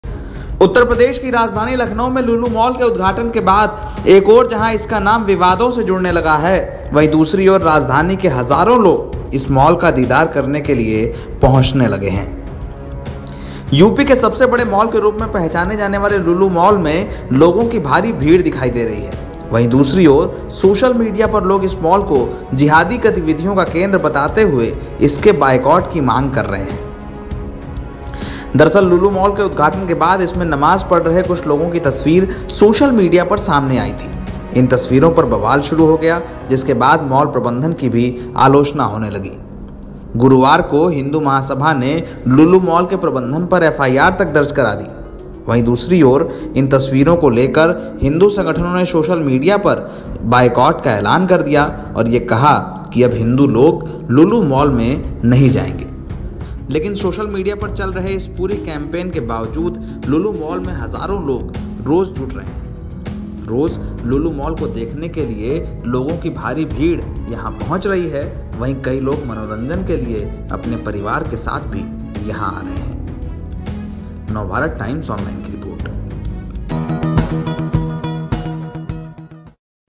Lulu Mall Lucknow Controversy: Social Media पर Boycott की मांग के बीच मॉल में जुट रहे हजारों लोग
लखनऊ में लुलु मॉल के उद्घाटन के बाद एक ओर जहां इससे कई विवादों का नाम जुड़ने लगा है, वहीं दूसरी ओर राजधानी के हजारों लोग रोज इस मॉल का दीदार करने के लिए आ रहे हैं। यूपी के सबसे बड़े मॉल के रूप में पहचाने जाने वाले लुलु मॉल में लोगों की भारी भीड़ दिख रही है। वहीं दूसरी ओर सोशल मीडिया पर लोग इस मॉल को जिहादी गतिविधियों का केंद्र बताते हुए इसके बायकॉट की मांग कर रहे हैं। दरअसल लुलु मॉल के उद्घाटन के बाद इसमें नमाज पढ़ रहे कुछ लोगों की तस्वीर सोशल मीडिया पर सामने आई। इन तस्वीरों पर बवाल शुरू हो गया, जिसके बाद मॉल प्रबंधन की भी आलोचना होने लगी। दूसरी ओर ट्विटर और फेसबुक पर कुछ हिंदू संगठनों ने कहा कि लुलु मॉल का बायकॉट किया जाना चाहिए, जिसका कई लोगों ने समर्थन भी किया। इसके बावजूद लखनऊ में लुलु मॉल को देखने के लिए हर रोज बड़ी संख्या में लोग पहुंच रहे हैं। इसके अलावा तमाम लोग यहां पर खरीदारी और मनोरंजन के लिए अपने परिवार के साथ भी आ रहे हैं।